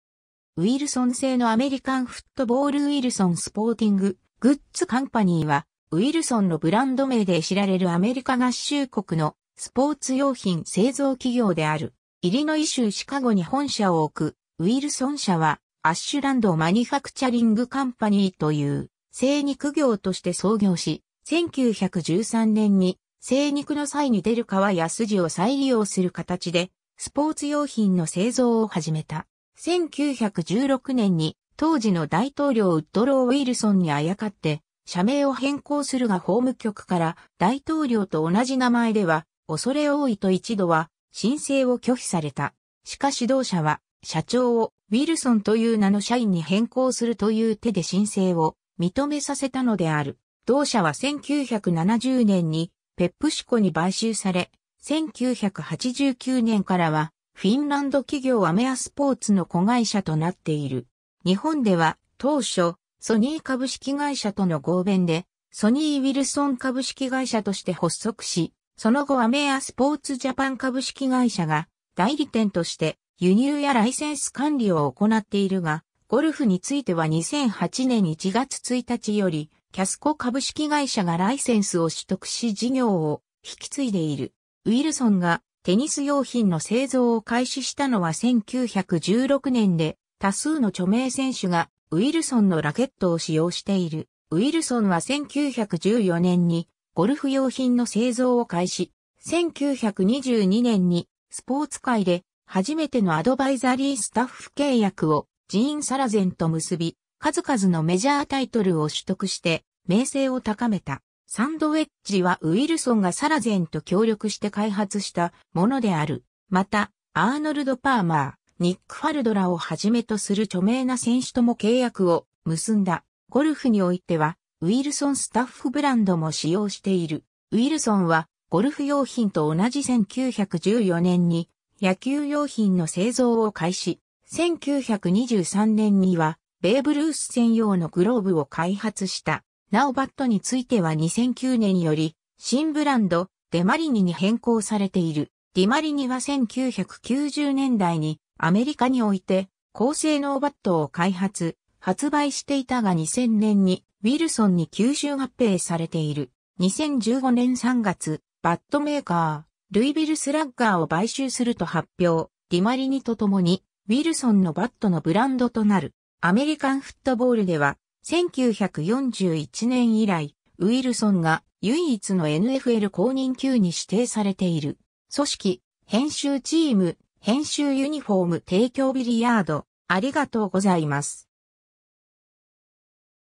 ウイルソン・スポーティング・グッズ
ウイルソン製のアメリカンフットボール ウイルソン・スポーティング・グッズ・カンパニー（Wilson Sporting Goods Company）は、ウイルソン（Wilson）のブランド名で知られるアメリカ合衆国のスポーツ用品製造企業である。
 イリノイ州シカゴに本社を置くウイルソン社は「アッシュランド・マニファクチャリング・カンパニー」という精肉業として創業し、1913年に精肉の際に出る皮やスジを再利用する形でスポーツ用品の製造を始めた。
1916年に当時の大統領ウッドロウ・ウィルソンにあやかって社名を変更するが法務局から「大統領と同じ名前では畏れ多い」と一度は申請を拒否された。
しかし同社は「社長を「ウィルソン」という名の社員に変更する」という手で申請を認めさせたのである。
同社は1970年にペプシコに買収され、1989年からはフィンランド企業アメアスポーツの子会社となっている。
 日本では、当初、ソニー株式会社との合弁で、ソニーウィルソン株式会社として発足し、その後アメアスポーツジャパン株式会社が代理店として輸入やライセンス管理を行っているが、ゴルフについては2008年1月1日よりキャスコ株式会社がライセンスを取得し事業を引き継いでいる。
 ウイルソンがテニス用品の製造を開始したのは1916年で、多数の著名選手がウイルソンのラケットを使用している。
 ウイルソンは1914年にゴルフ用品の製造を開始。
1922年にスポーツ界で初めてのアドバイザリースタッフ契約をジーン・サラゼンと結び、数々のメジャータイトルを取得して名声を高めた。
サンドウェッジはウイルソンがサラゼンと協力して開発したものである。
また、アーノルド・パーマー、ニック・ファルドらをはじめとする著名な選手とも契約を結んだ。
ゴルフにおいては、ウイルソン・スタッフブランドも使用している。
 ウイルソンはゴルフ用品と同じ1914年に野球用品の製造を開始。
1923年にはベーブ・ルース専用のグローブを開発した。
 なおバットについては2009年より新ブランド「DeMARINI（ディマリニ）」に変更されている。
ディマリニは1990年代にアメリカにおいて高性能バットを開発・発売していたが2000年にウィルソンに吸収合併されている。
 2015年3月、バットメーカー・ルイビルスラッガーを買収すると発表、ディマリニとともにウィルソンのバットのブランドとなる。
 アメリカンフットボールでは、1941年以来、ウイルソンが唯一のNFL公認球に指定されている。
 組織[編集] チーム[編集] ユニフォーム提供 ビリヤード